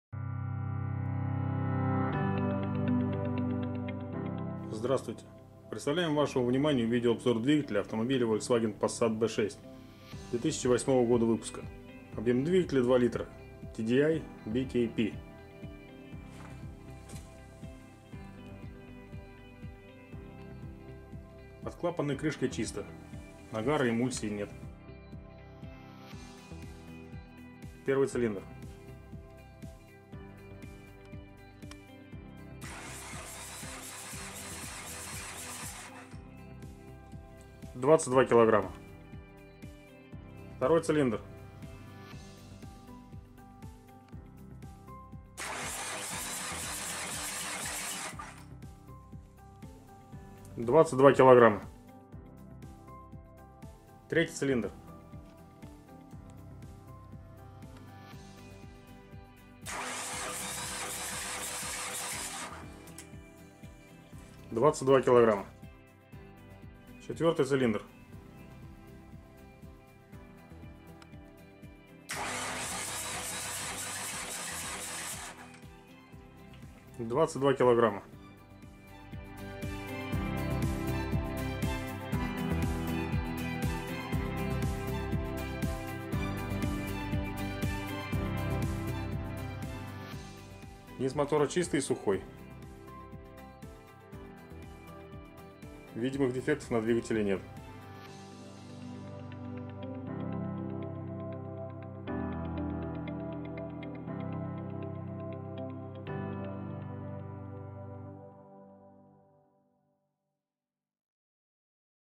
Двигатель VW для Passat [B6] 2005-2010
Описание двигателя Двигатель VW для Passat [B6] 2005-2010
2.0Л. 16V ДИЗЕЛЬ ТУРБО 2008Г. КОМПРЕССИЯ: 1Ц:22 2Ц:22 3Ц:22 4Ц:22 ХОЛОДНАЯ КОМПРЕССИЯ ЕСТЬ ВИДЕООБЗОР ДВИГАТЕЛЯ И ЗАМЕРА КОМПРЕССИИ КОНТРАКТНЫЙ ЕСТЬ СОПРОВОДИТЕЛЬНЫЙ ПАКЕТ ДОКУМЕНТОВ ГТД TDI BKP 03G100037K 03G100098NX